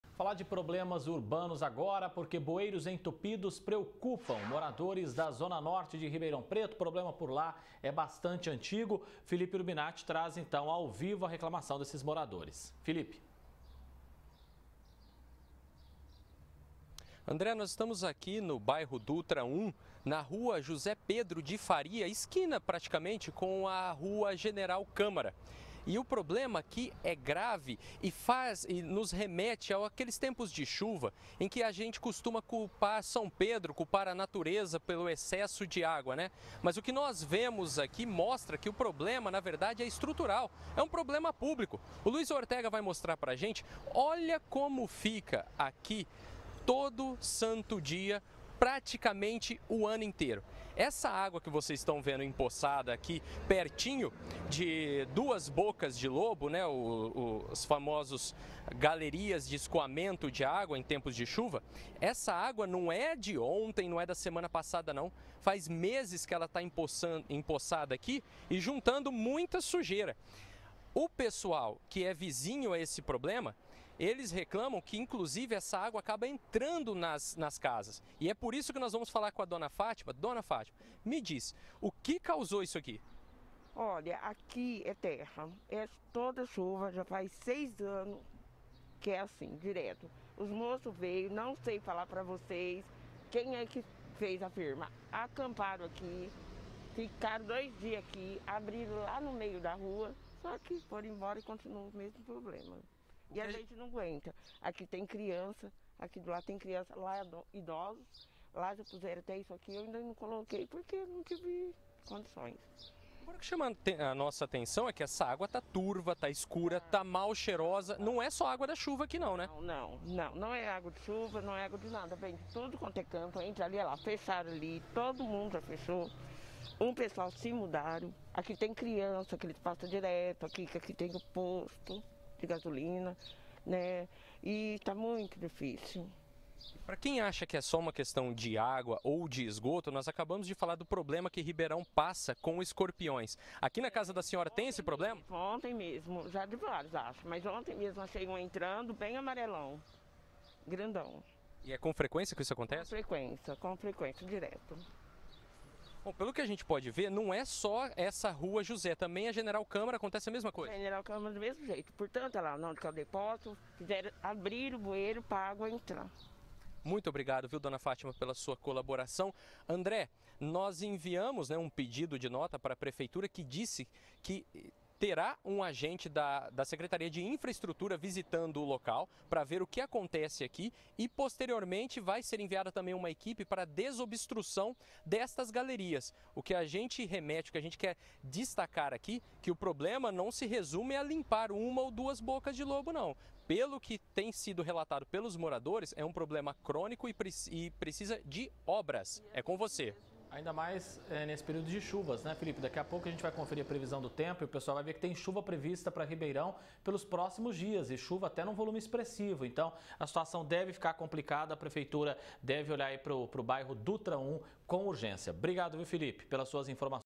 Bueiros entupidos preocupam vizinhança da zona norte de RP - Jornal da Clube (10/11/2022)
O problema acontece no Bairro Dutra 1 e já se perdura há mais de um ano.